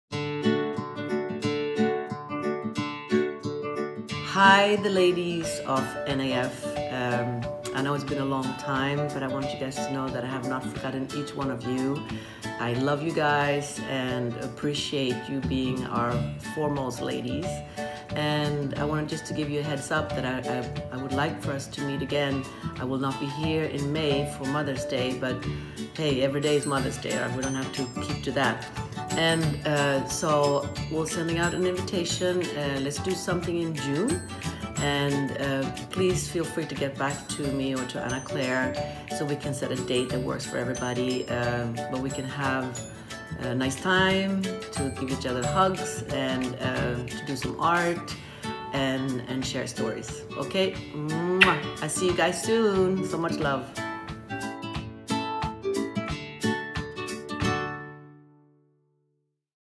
Special Message from Cecilia Rodhe to the Mothers of Noah's Arc, May 2018
Special Message from Cecilia Rodhe, Co-Founder and President of Noah's Arc Foundation to the Mothers of Noah's Arc with a special invitation to join the Spring Gathering!